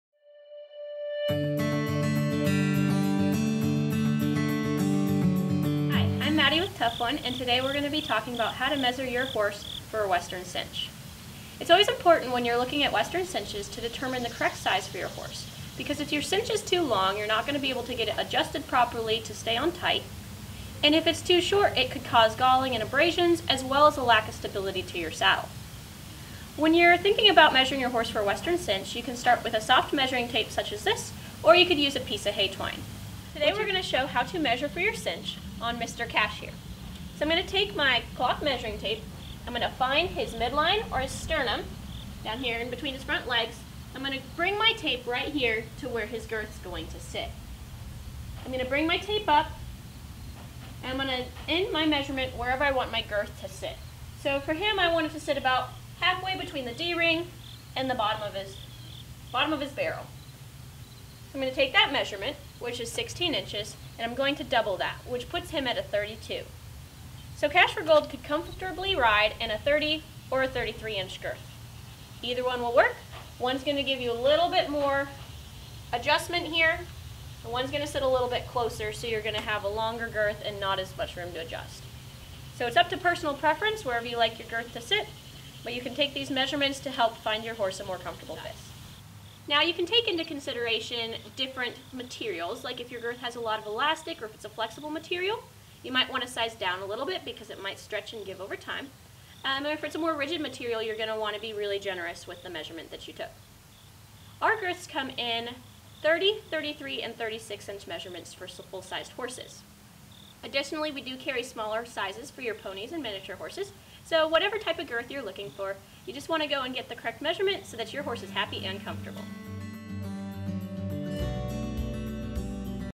How To: Measure for a Western Cinch
It's very important to use the correct size cinch for your Western saddle: if the cinch is too short, your horse can get abrasions. If it's too long, it will hold the saddle in place, but there will be excess material under your leg. Tough1 Sales Counselor Maddie shows you how to measure for the appropriate sized cinch to help you choose the right Tough1 Western cinch.

Tough1 offers a wide variety of materials, styles and sizes for Western cinches for ponies and horses. Search for them at your favorite tack dealer.